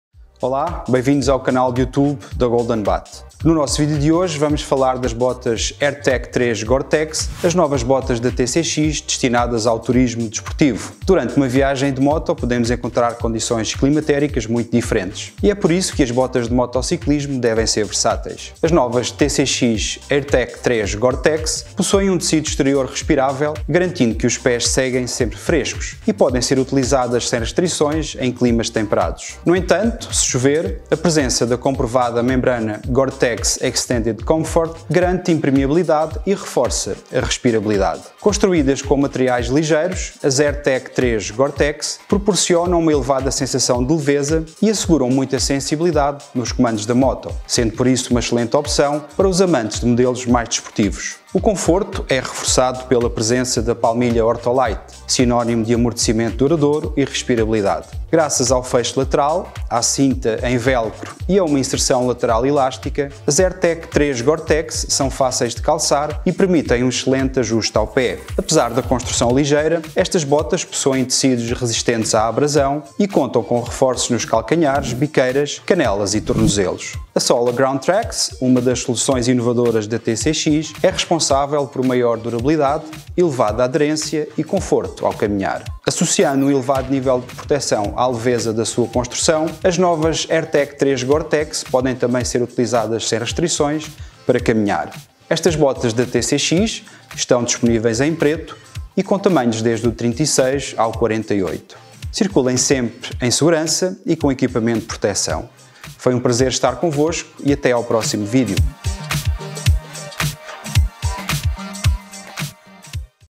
TCX AIRTECH 3
As novas botas de moto destinadas ao turismo de estrada, as TCX Airtech 3 Gore-tex, elevam para um novo patamar a sensação de leveza e a sensibilidade ao conduzir a sua moto, e são perfeitas mesmo para aqueles que gostam de rolar em ritmos mais elevados.